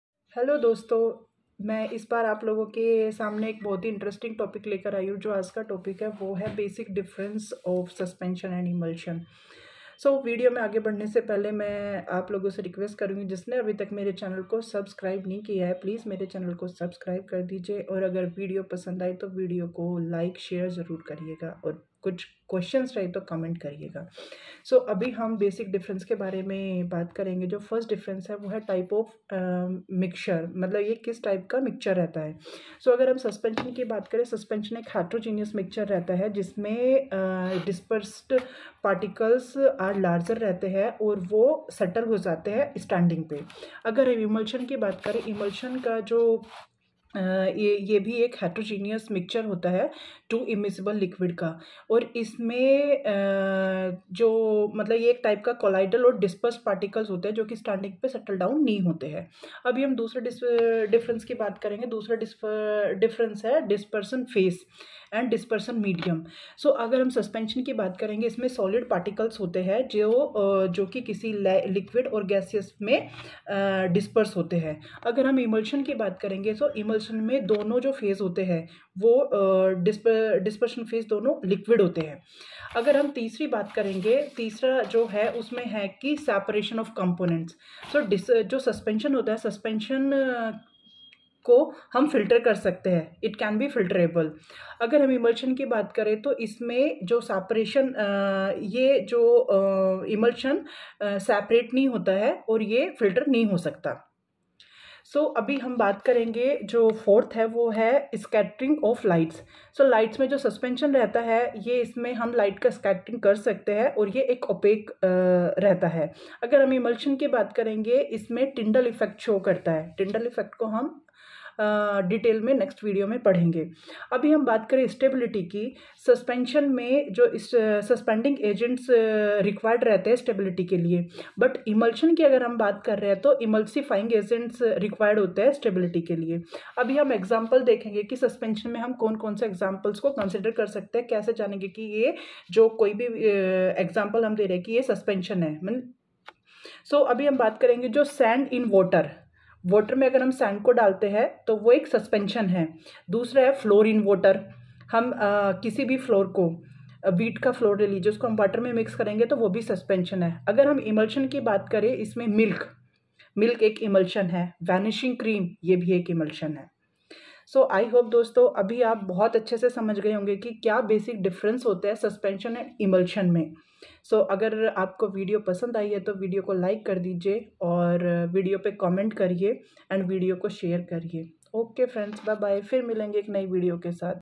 what is difference between suspension and emulsion #@lifeLong28
difference between suspension and emulsion # type of mixture
disperesd phase
seperation of components
scattering of light